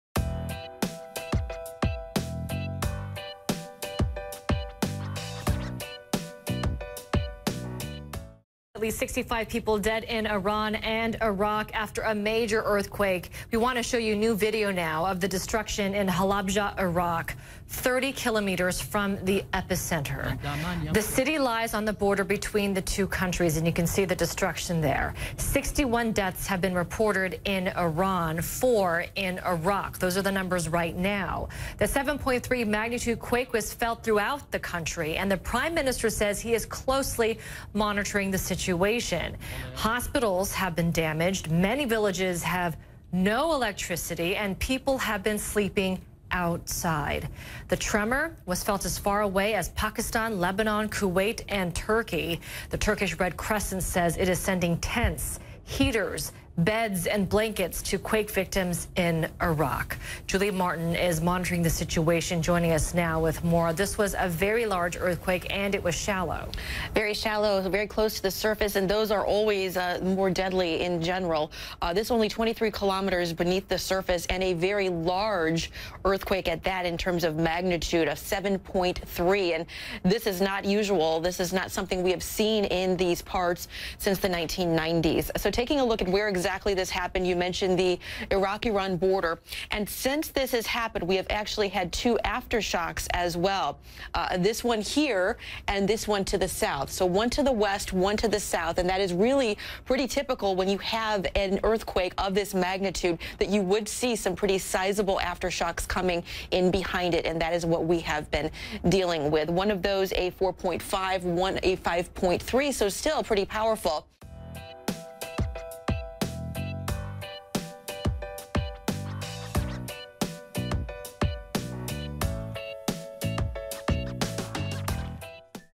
EARTHQUAKE AFTERSHOCKS SHAKE IRAQ-IRAN BORDER | WORLD NEW TODAY
A 7.3 magnitude earthquake along the Iraq-Iran border dozens. CNN's Amara Walker and Julie Martin have the latest.